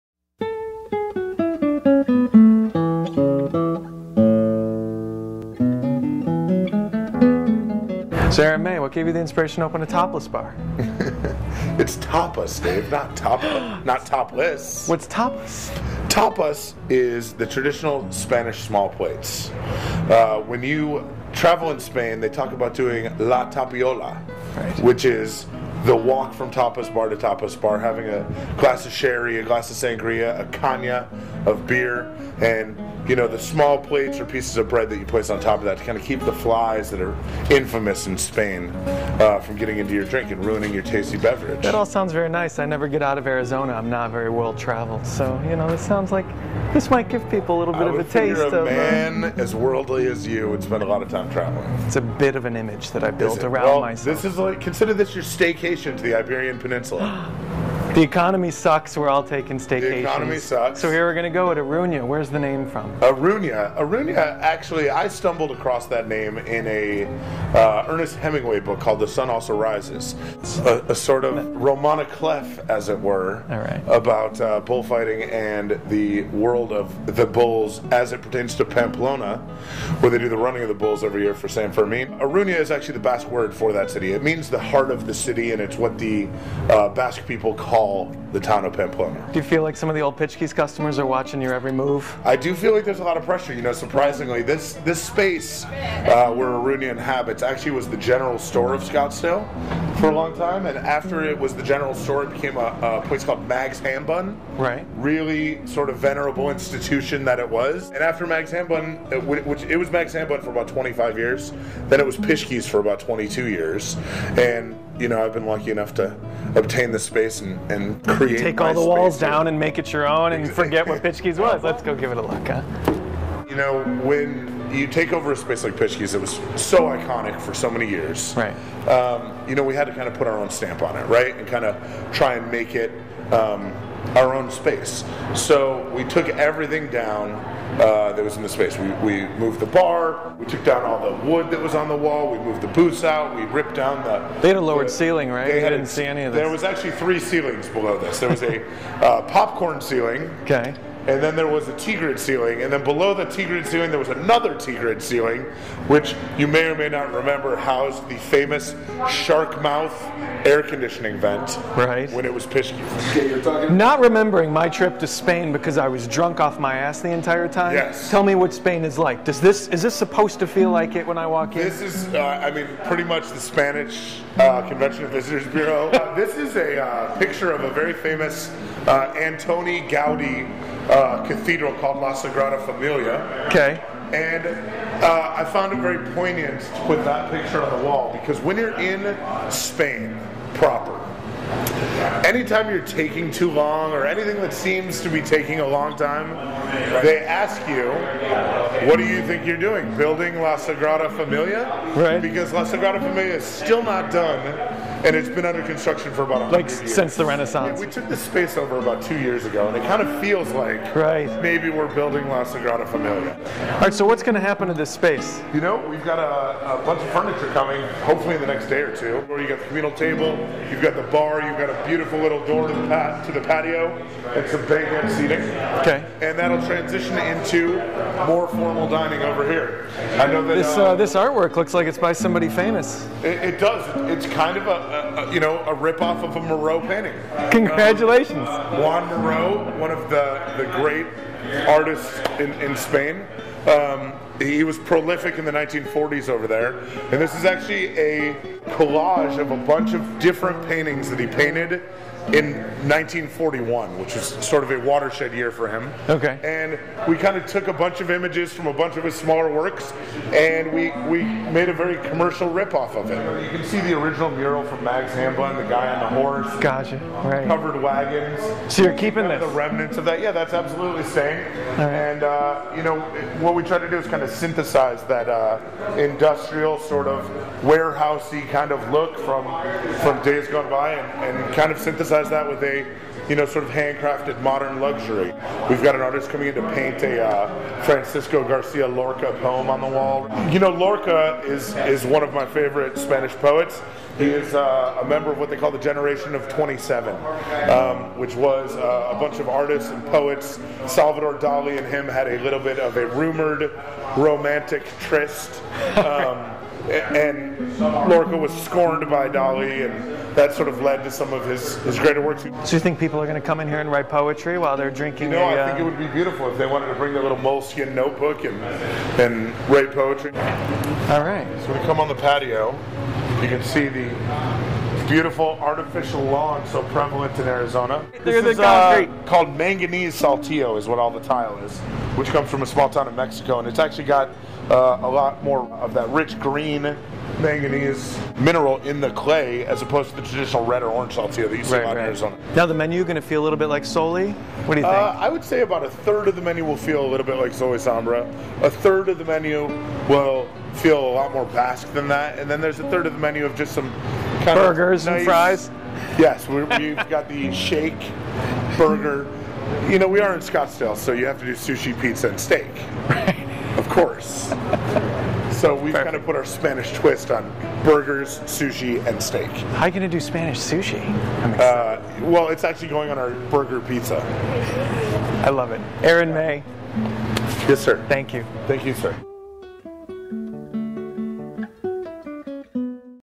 TOUR THROUGH SPAIN
This Friday, Chef Aaron May will begin serving test meals to friends and family at Iruña, his new production with longtime partner Quinn Goldsberry. You could say that Iruña (pronounced something like I ruin ya) provides May a chance to the get back to doing what he loves most—cooking Spanish food with French discipline in a tightly run kitchen to a mix of food-conscious and scene-conscious. Unfortunately, the town we live in runs a little heavy on the latter, but that didnt stop May from trying to open their minds with razor clams or a killer bocadillo de chorizo at his first big AZ venture—Sol Y Sombra in North Scottsdale. Although Sol went down with the economy, May found ways to open two locations of the now-popular breakfast joint, Over Easy, a diner called 18 Degrees, and downtown Scottsdale bars The Lodge and Mabels on Main.

Flippin pancakes at Over Easy cant be exciting for the chef who opened Deseo at the Westin Kierland Resort with Chef Douglas Rodriguez, but it certainly pays the bills. Mabels was never a culinary venture for May, but it certainly serves some upbeat bar snacks that showcase some of his deft touches (Oysters Rockefeller anyone?). Iruñas menu, on the other hand, will kick off with 35 small plates. May says a third of them come straight from the menu at Sol Y Sombra, another third was inspired by tapas from the Basque region of Spain, and the final third is made up of simple crowd-pleasers tailored to appease the town we live in.

Is May admitting that people dont really have the palate to assess a good razor clam? We dont think so. Like Iruña itself, May is trying to educate through osmosis. Antoni Gaudi architecture, Federico Garcia Lorca poetry, Joan Miro paintings—theyre all well-researched pieces of the Iruña equation. Watch the video interview here to see how it all comes together. Now its up to the menu. We just hope there's a sushi-topped burger pizza on that thing.